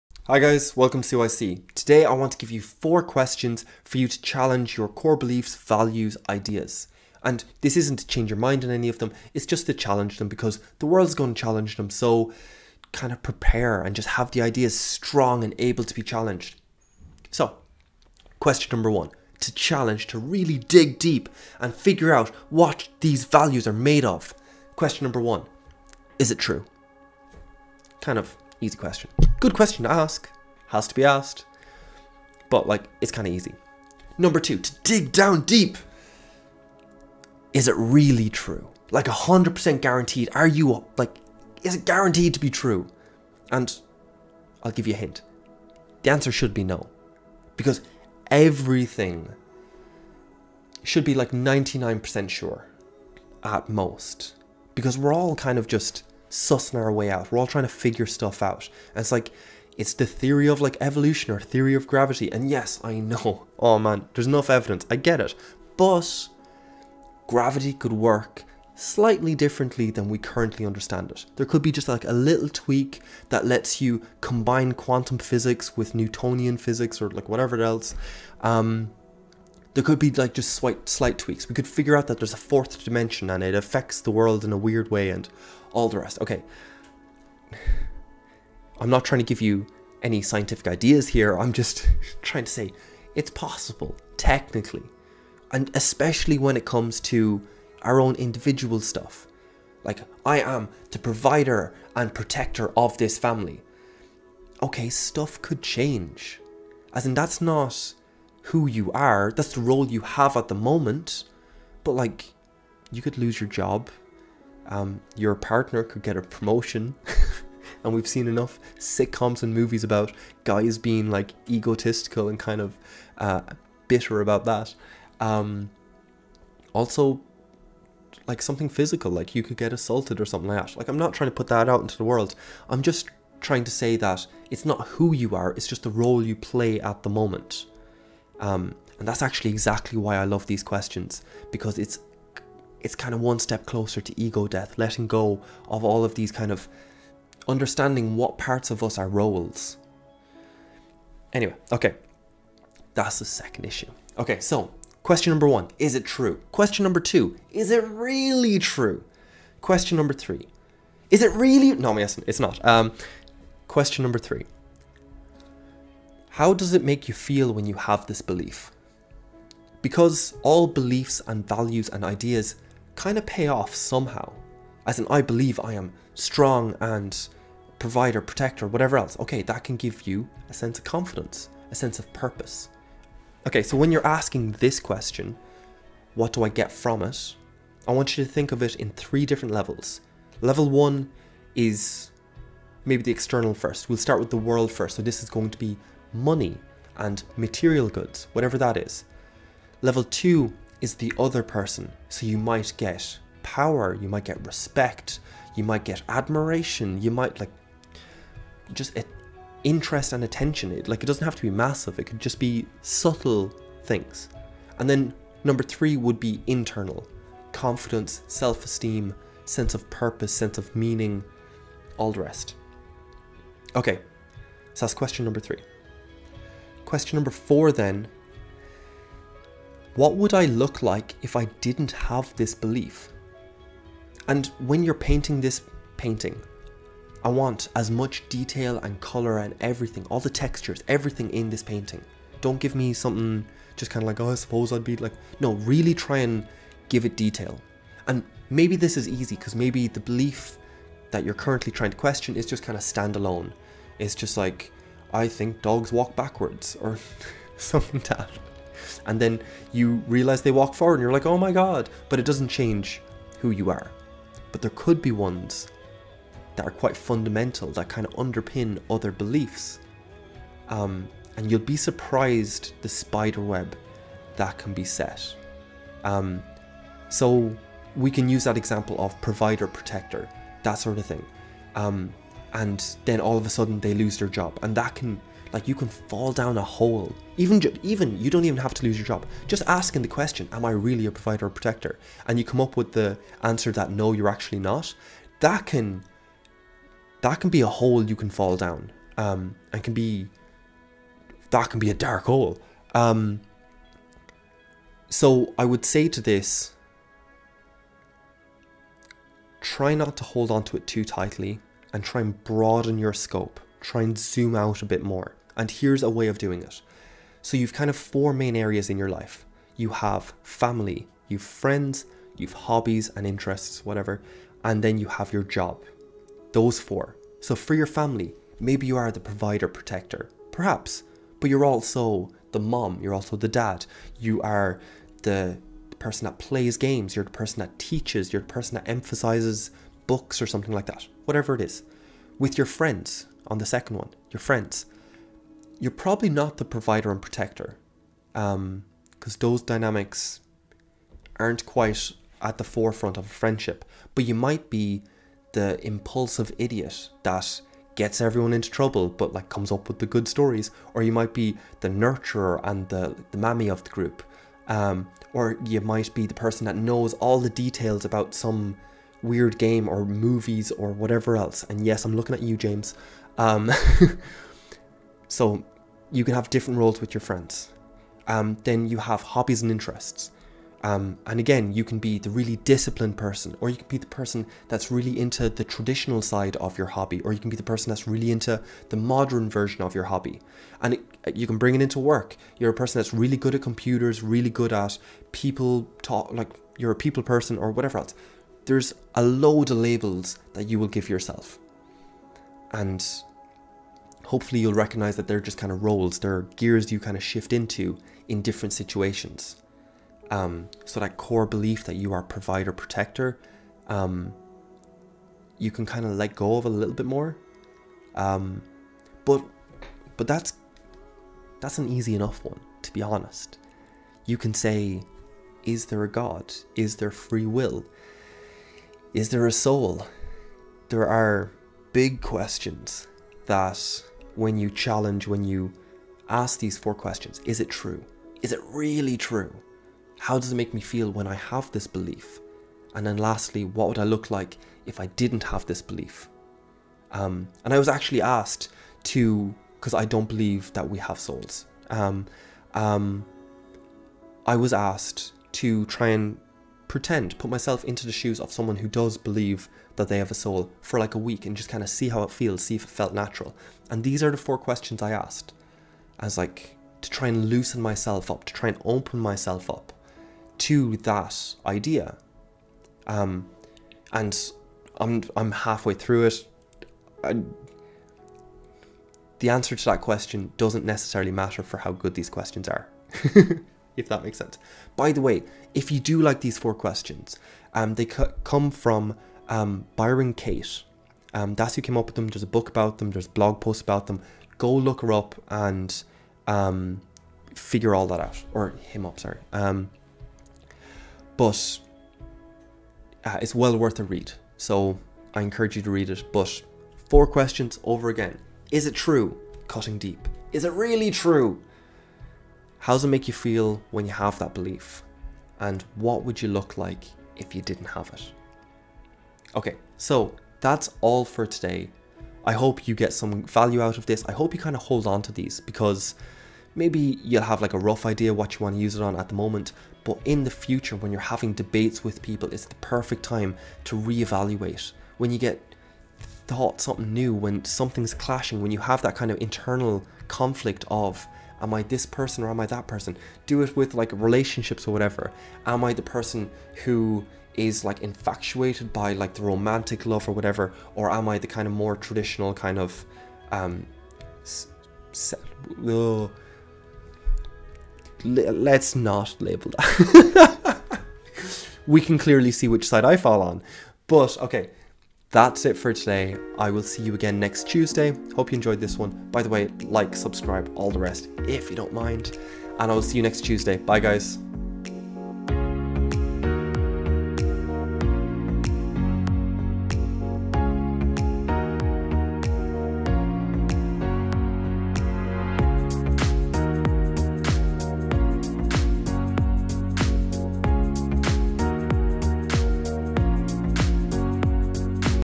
Are your ideas being challenged? (4 questions strengthen your answers)
So last week we did 5 core values (link below), this week we're looking at how to examine and critiques values and ideas with 4 easy questions. These are the early steps in ego death as far as I can tell, but regardless, it's a very powerful tool to use to strengthen your argument, and to challenge others. I hope you enjoy the video.

As always, Thank you all for your support. see you again next Tuesday :) (roughly)